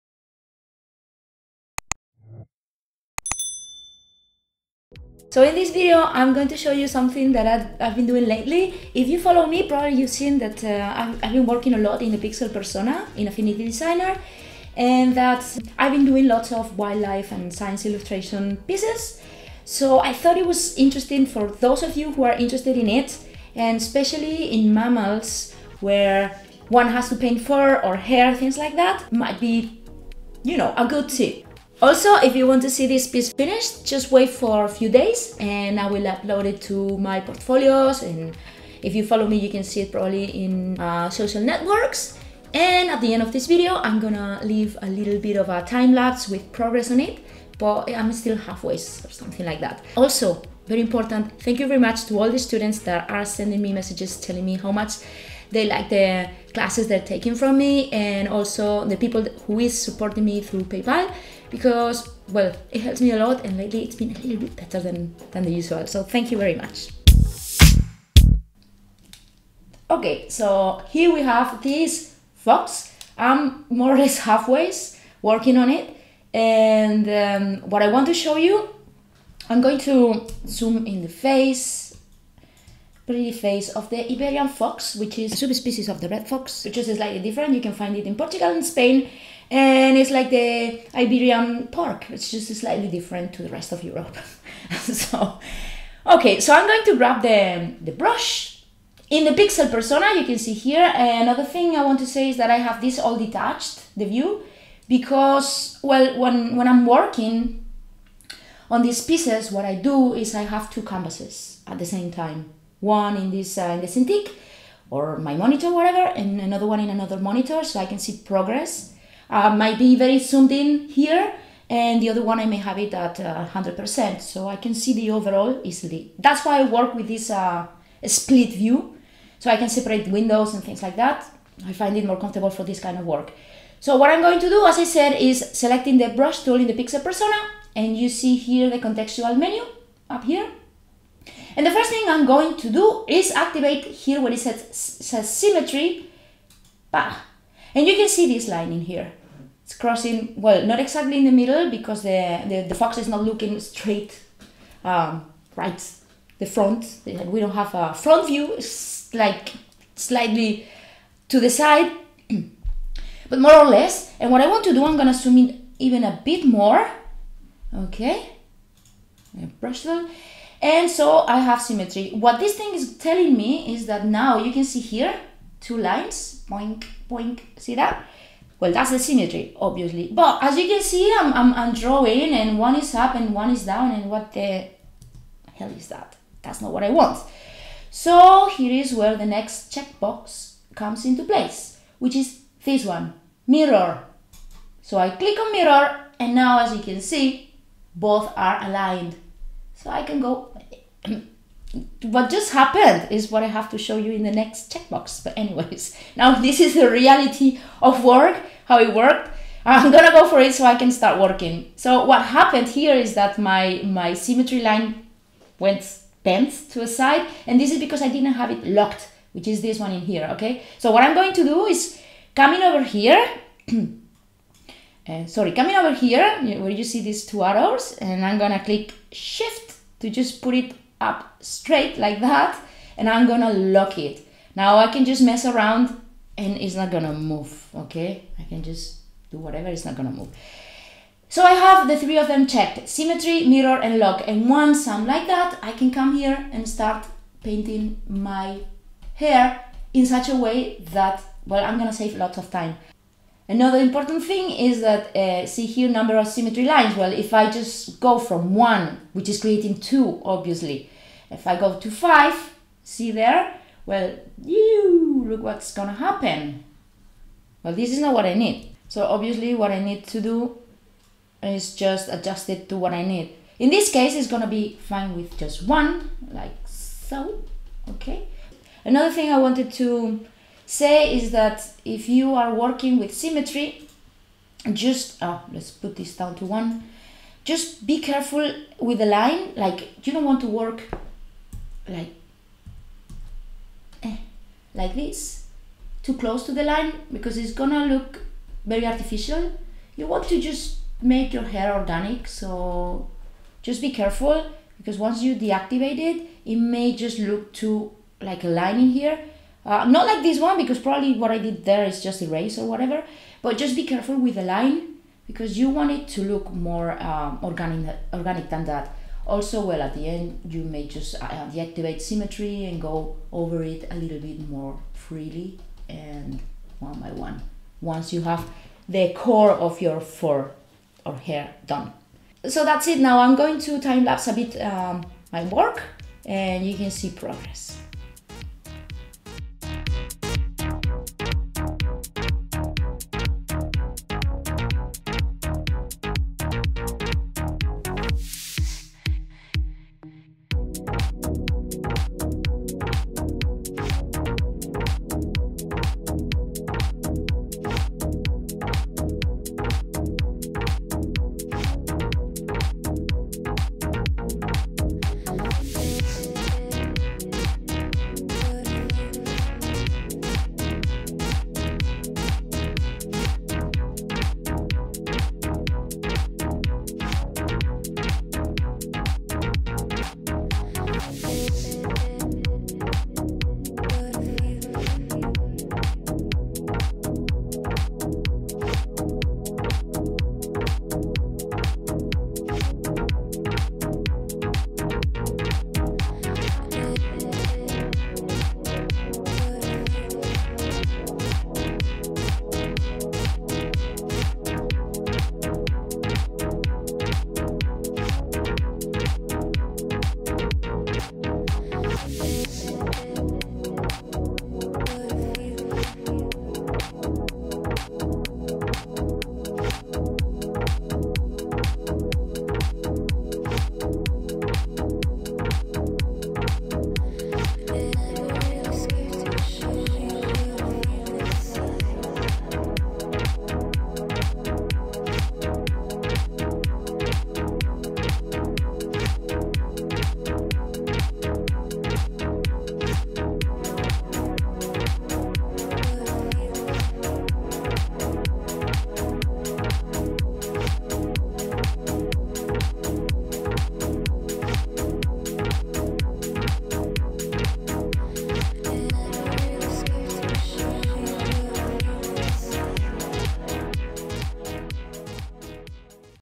Affinity Designer - Using Symmetry for Fur & Hair
Tip on how to speed up work when painting fur or hair in Affinity Designer. Using symmetry painting and its options. Time-lapse to see work progress at the end of video tip.